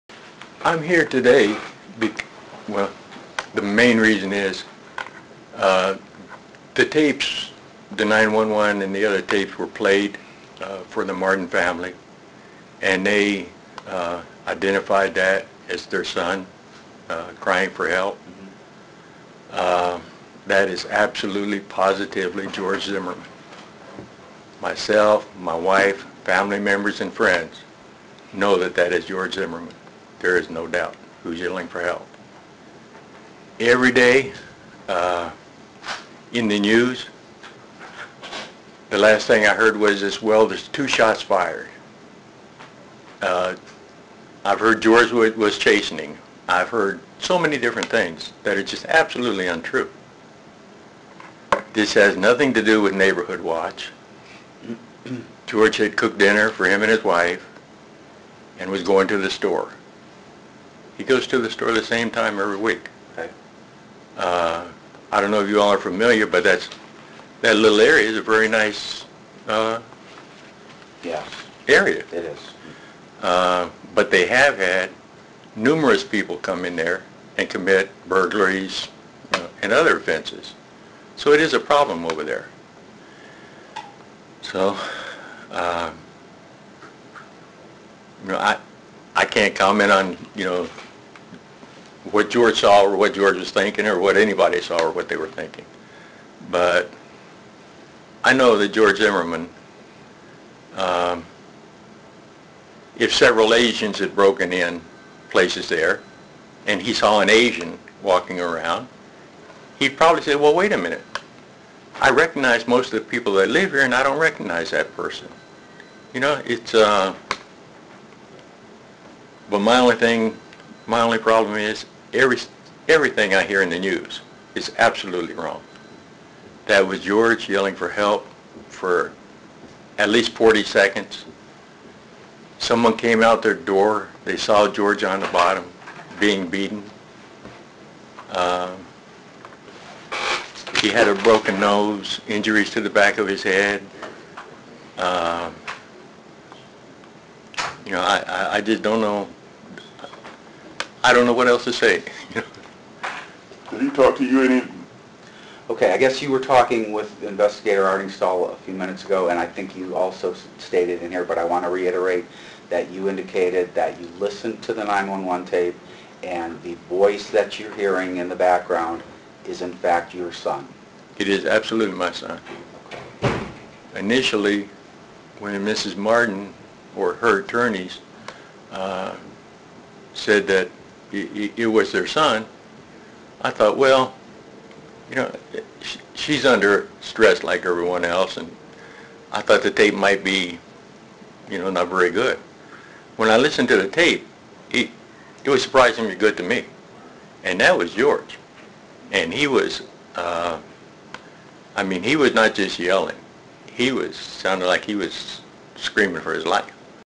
George Zimmerman's father says that is his son screaming for help
In a recorded interview with authorities George Zimmerman's father says that is his son screaming for help.